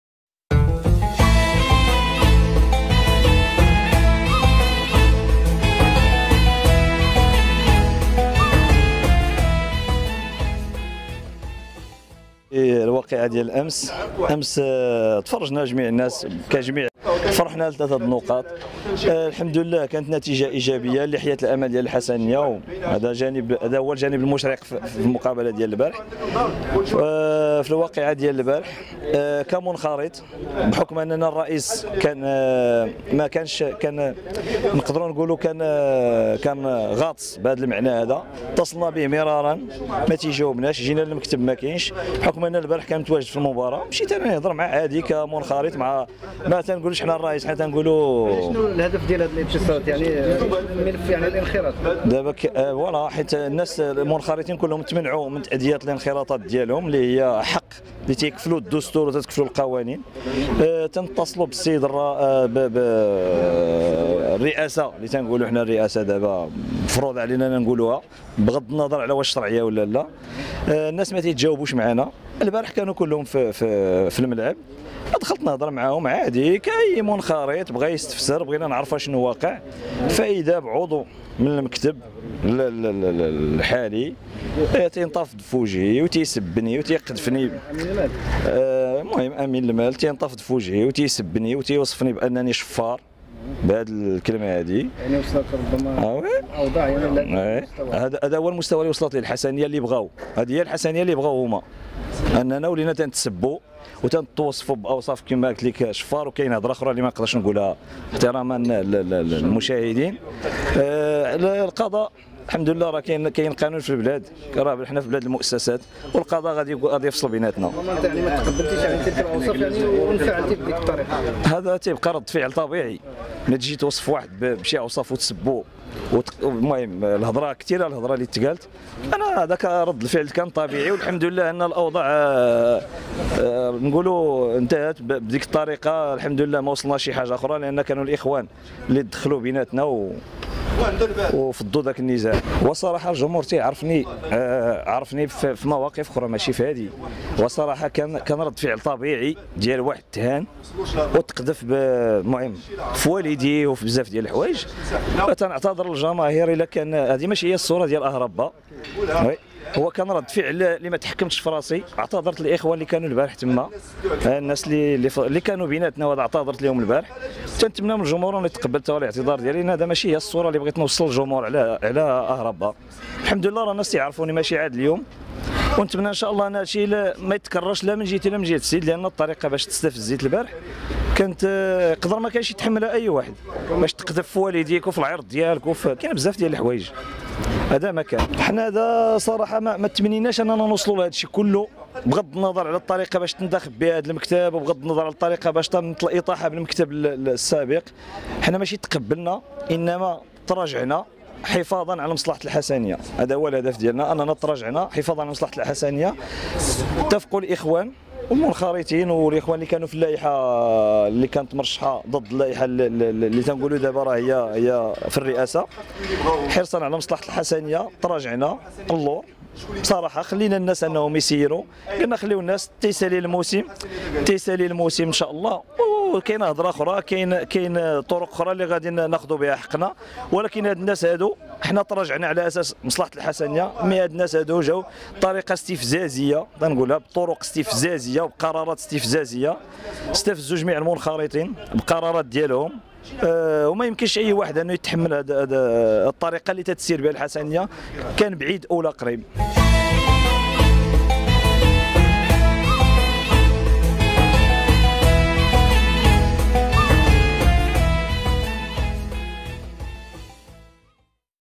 المنخرط بحسنية أكادير لي نوضها مع أمين المال يخرج عن صمته تعرضت للسب واتهمت أني شفار
كشف المنخرط بحسنية أكادير عن تفاصيل نزاعه مع أمين المال بالفريق.